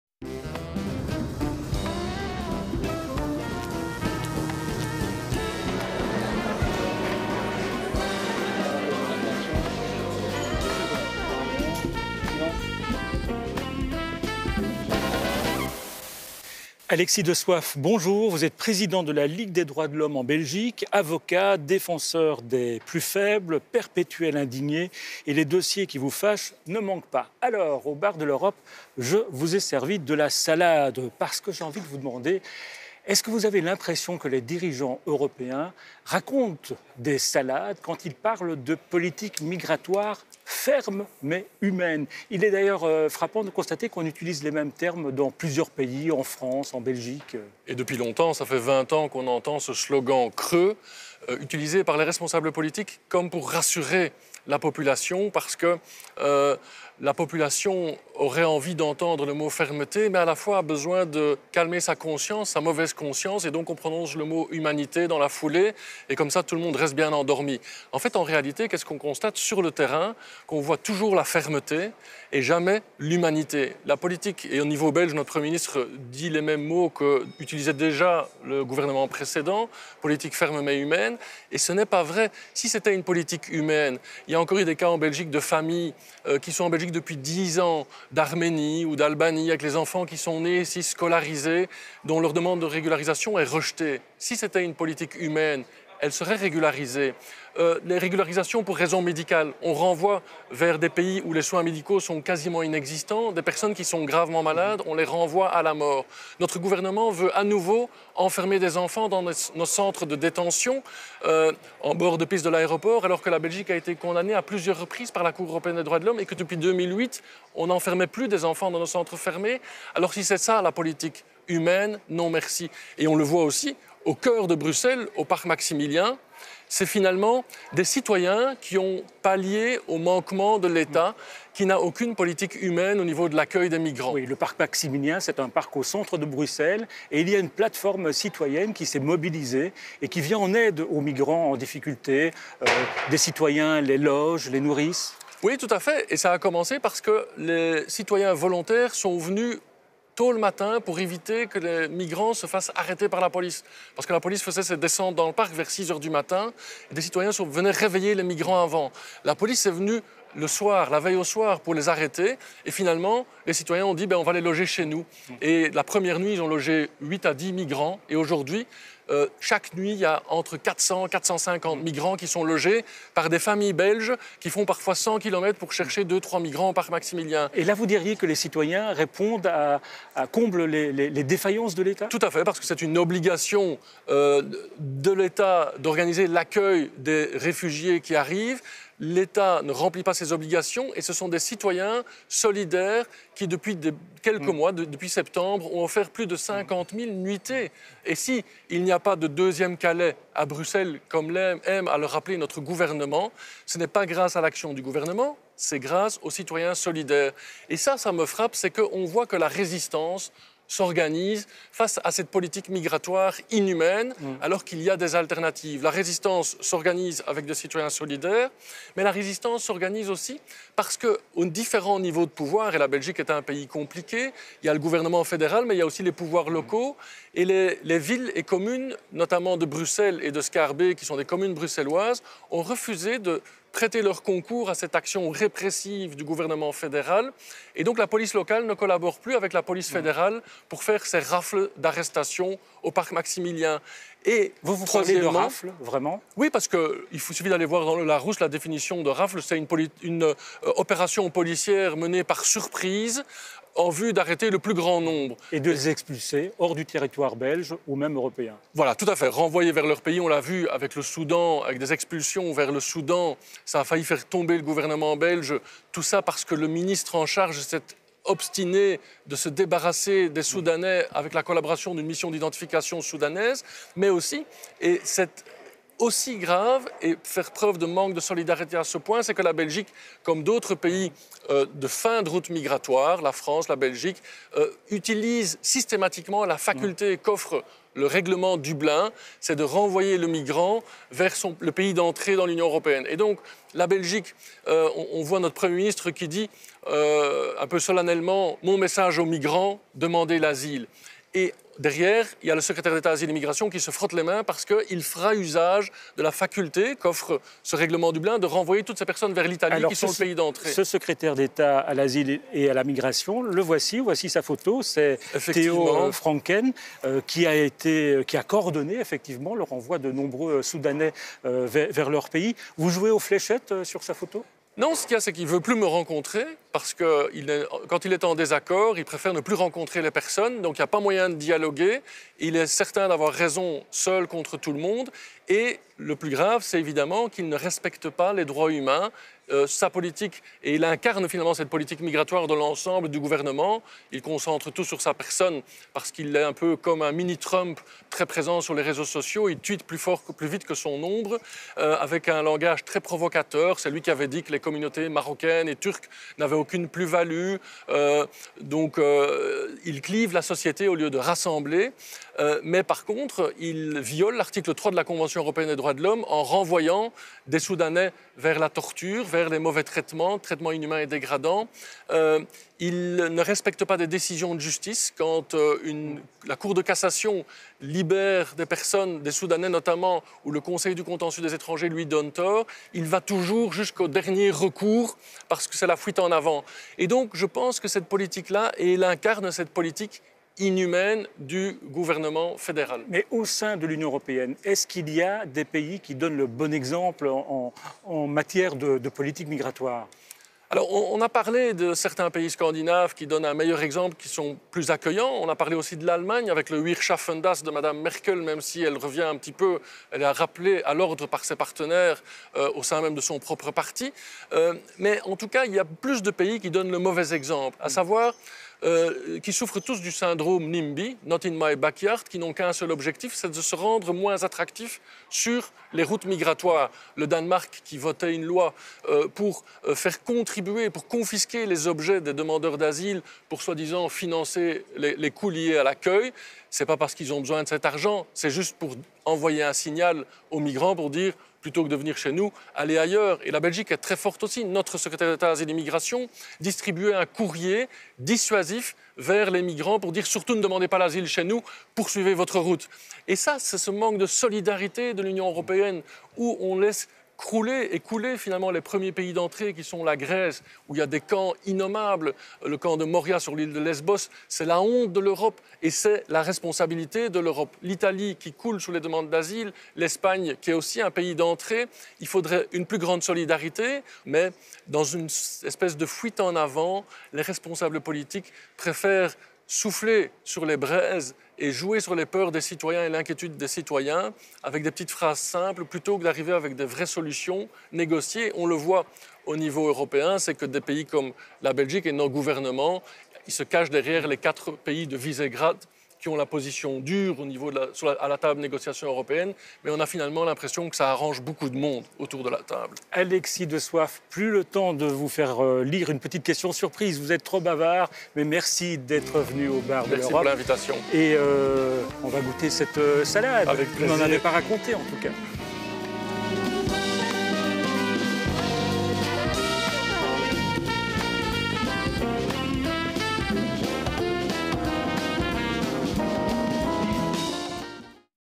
bardeleurope: Alexis Deswaef
C'est Alexis Deswaef qui s'accoude au Bar de l'Europe. Le Président de la Ligue des droits de l'homme en Belgique est un perpétuel indigné. Et il dénonce aujourd'hui les politiques migratoires prétendument "fermes mais humaines", slogan qui se répand dans plusieurs pays européens.